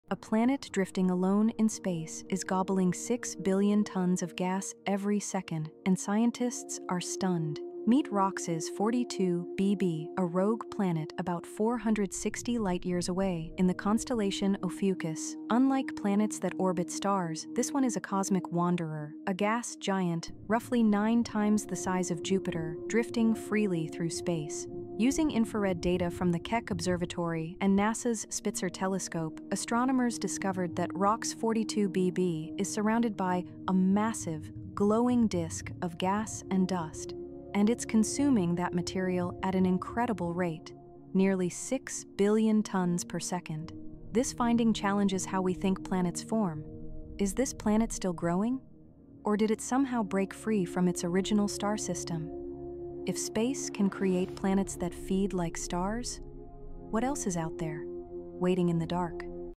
"The Planet That Eats Like a Star" - A Cosmic Monster#shorts
A planet drifting alone in space is devouring 6 billion tons of gas every second — and scientists are stunned.

Meet ROXs 42B b, a rogue planet about 460 light-years away in the constellation Ophiuchus. Unlike planets that orbit stars, this one is a cosmic wanderer — a gas giant roughly nine times the size of Jupiter, feeding like a young star.

Using infrared data from the Keck Observatory and NASA’s Spitzer Telescope, astronomers discovered that ROXs 42B b is surrounded by a massive glowing disk of gas and dust — and it’s consuming that material at an incredible rate.

Could this be a planet still growing… or one that escaped its star system?
If space can create worlds that feed like stars — what else is waiting in the dark?